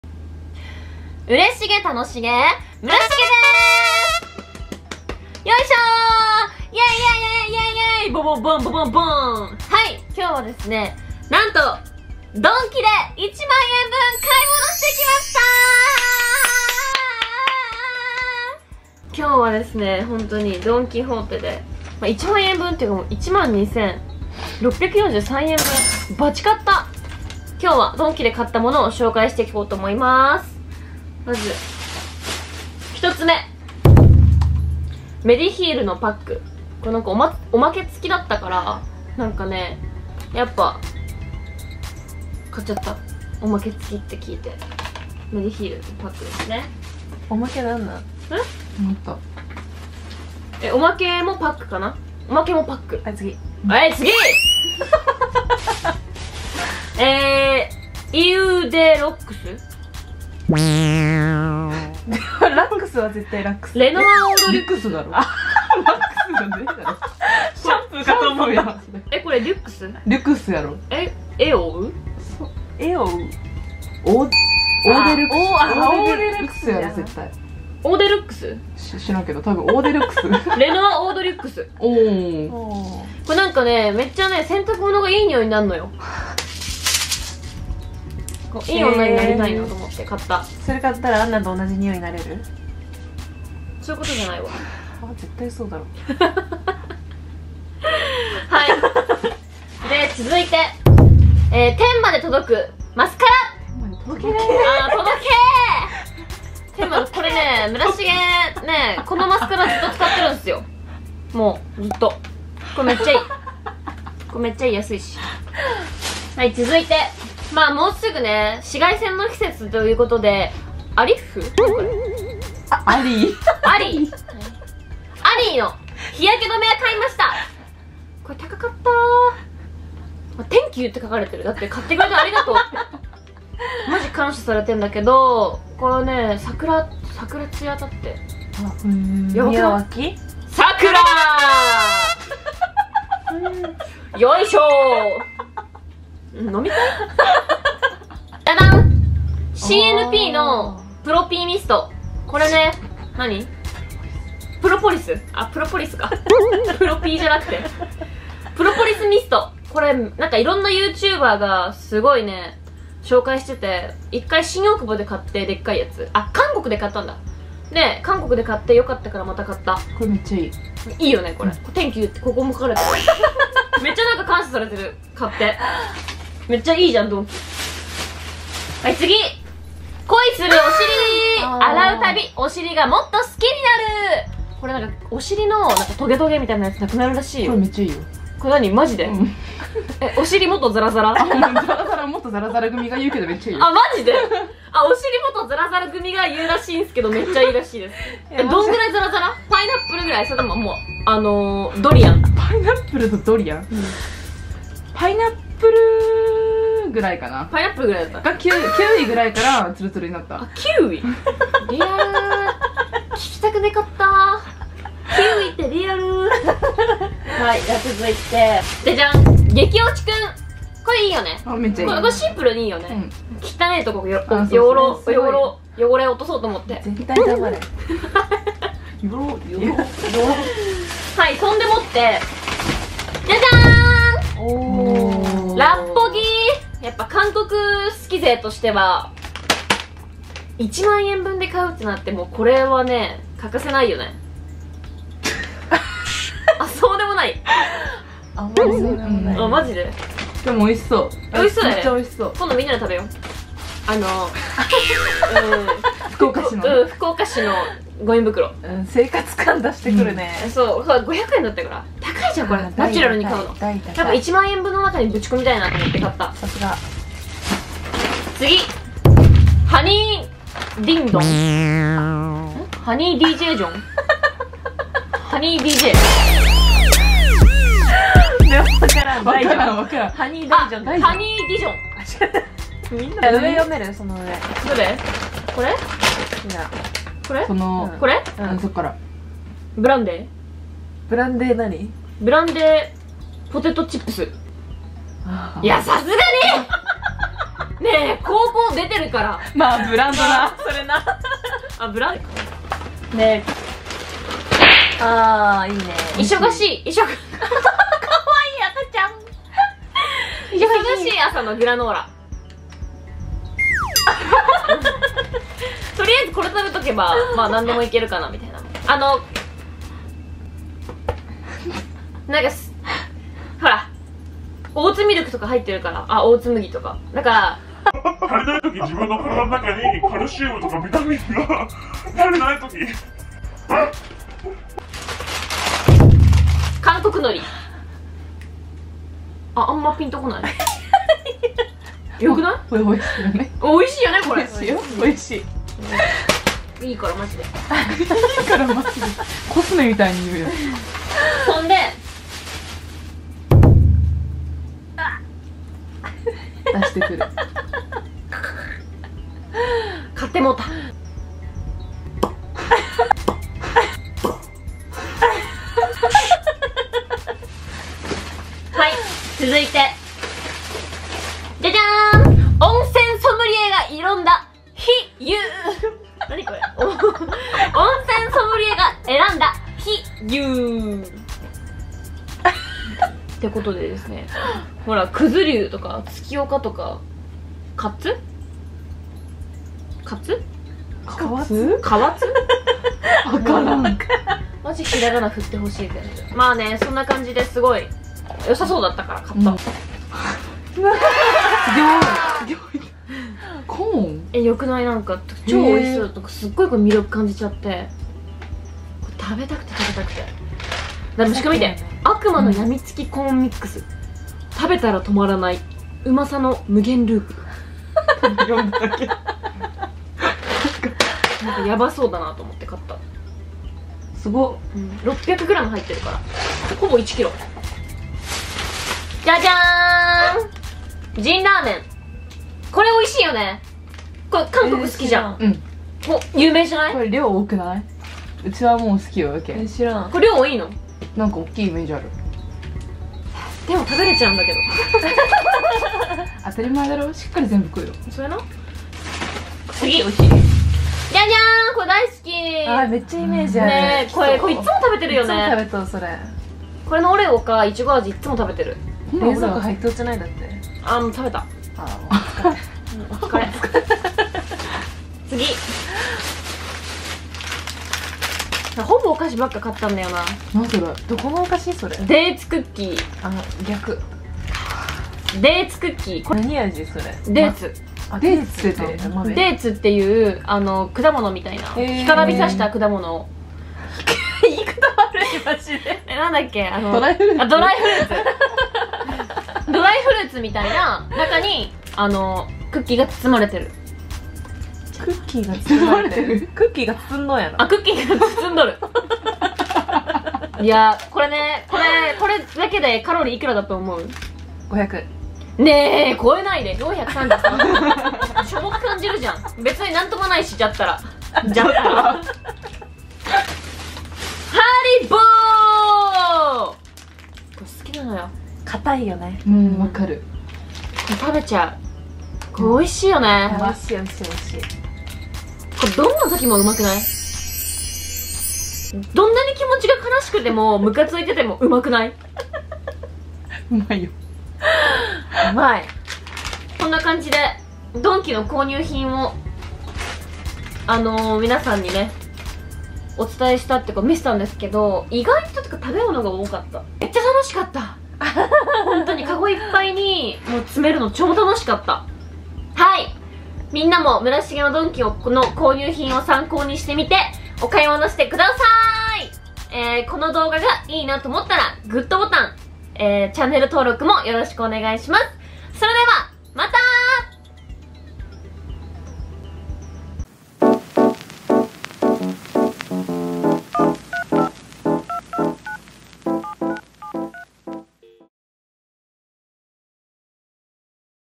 ドンキで爆買いしたけど、ただバカがバレただけだった
しげっこのみんなへ
ドンキで10000円分の爆買いしたんだけど漢字が読めなくて沈没しちゃった😦
みんなも村重の買ったもの参考にしてみてね〜
「日湯」は「ひとう」って読むらしいよ😭😭
自分で編集してるもんだから
間違いにも気づけなくて草☘️
とにかくみってっねー！！